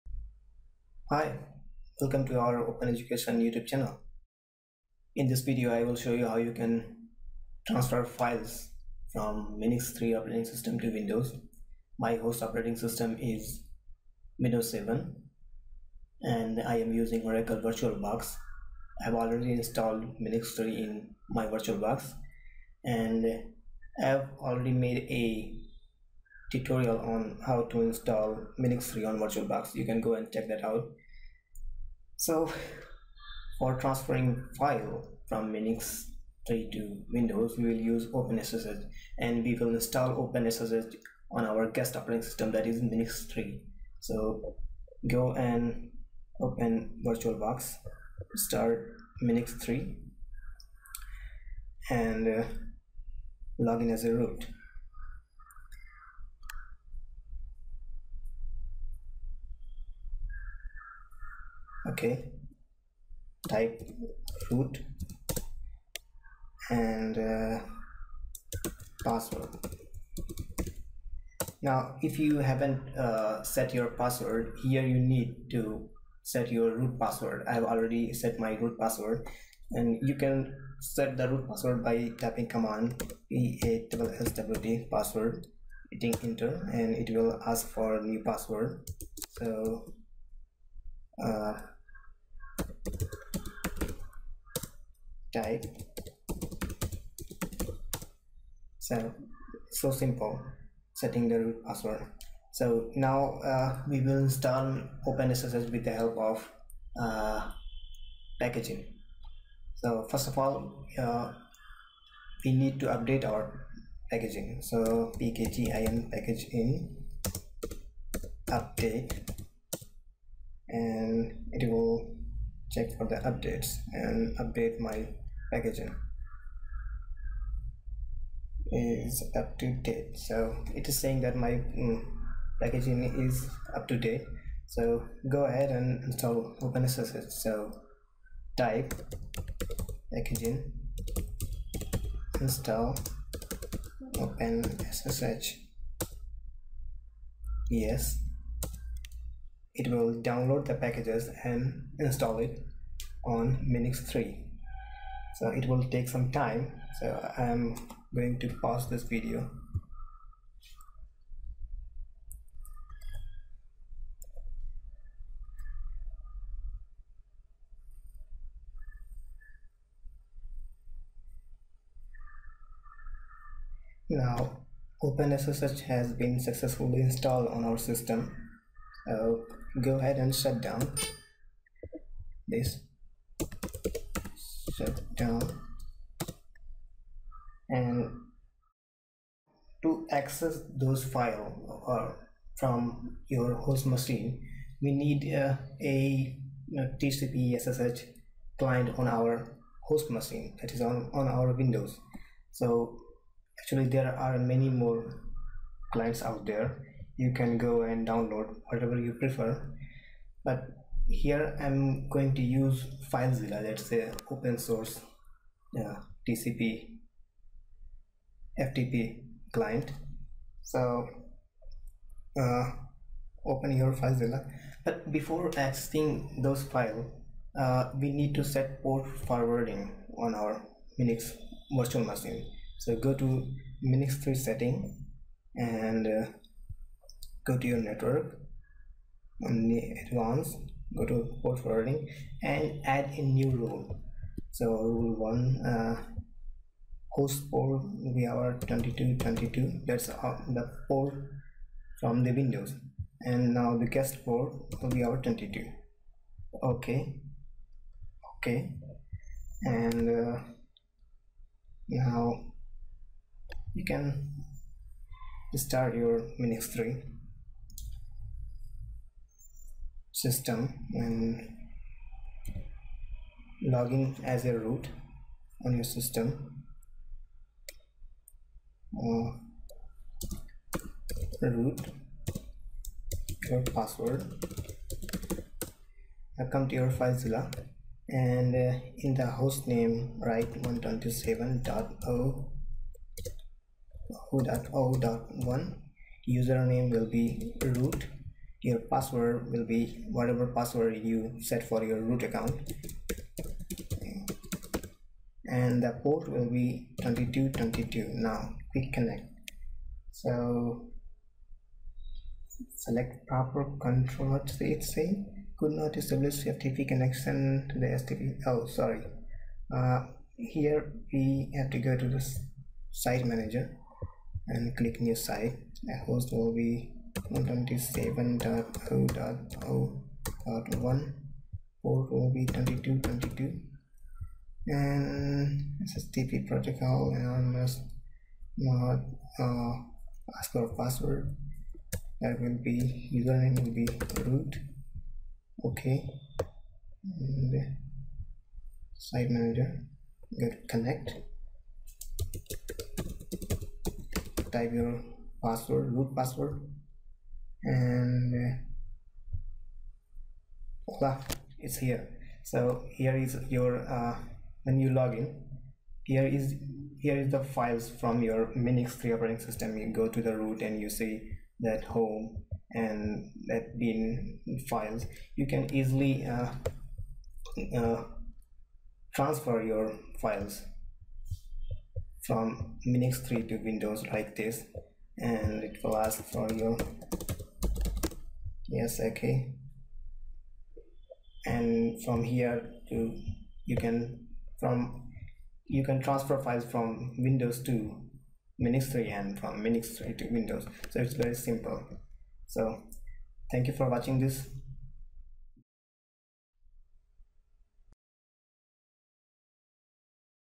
MINIX 3 / Windows File Transfer using Openssh
This video is about transferring/sharing files between MINIX 3 guest and Windows host operating system in VirtualBox using openssh.